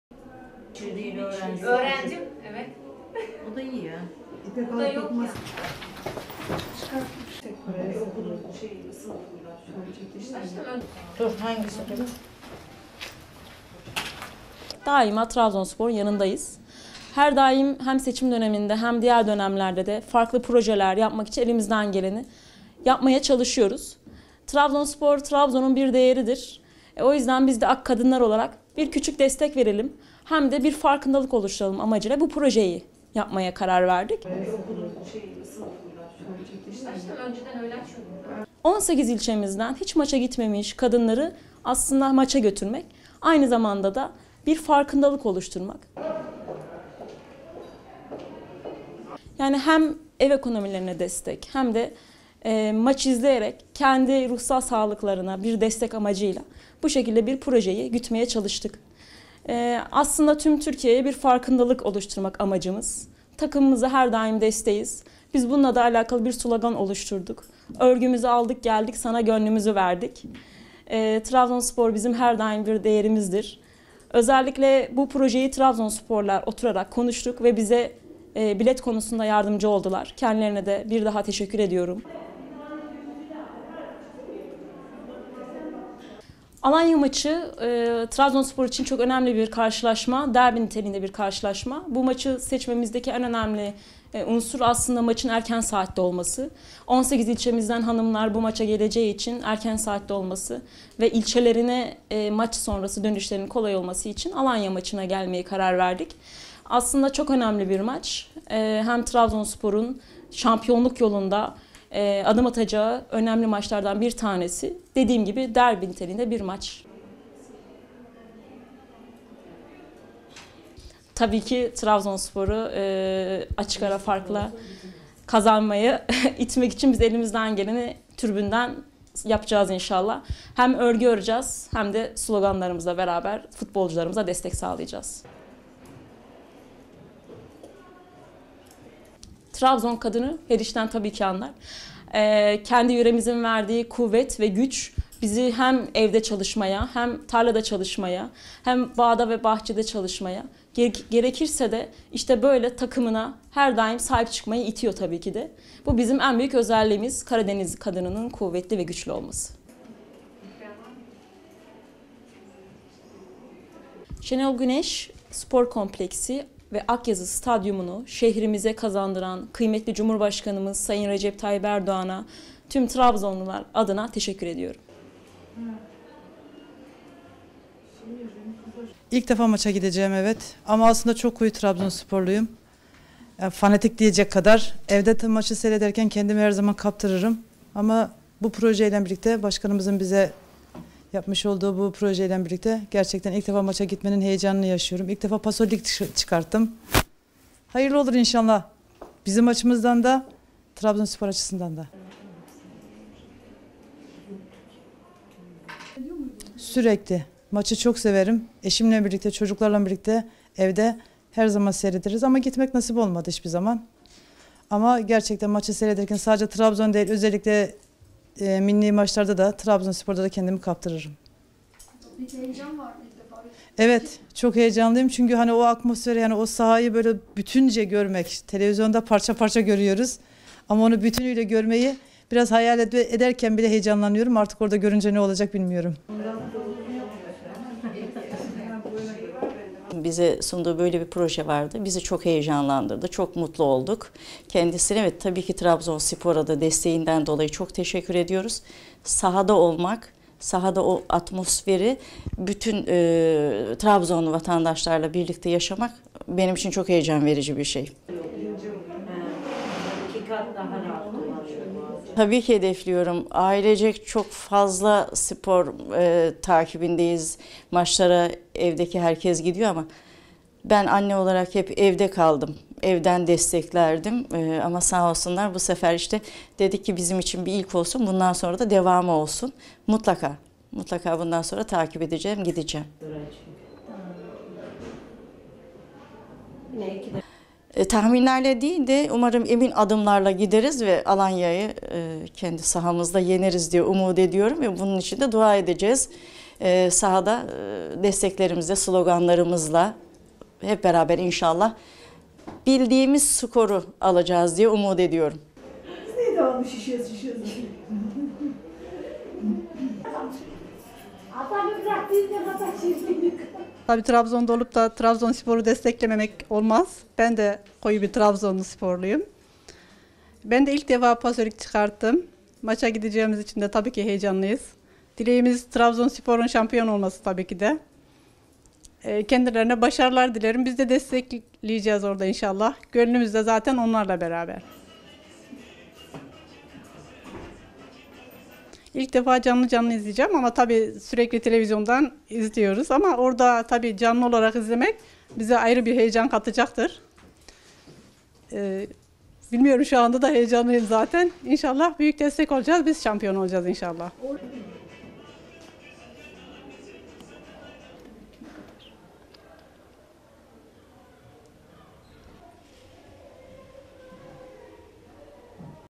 Trabzonsporlu Kadınlar
Daha önce stadyumda futbol karşılaşması izlememiş 40 kadın, Trabzonspor'un 10 Kasım Pazar günü sahasında Aytemiz Alanyaspor ile oynayacağı maçta hem takımlarına destek verecek hem de yörenin el sanatlarını işleyecek.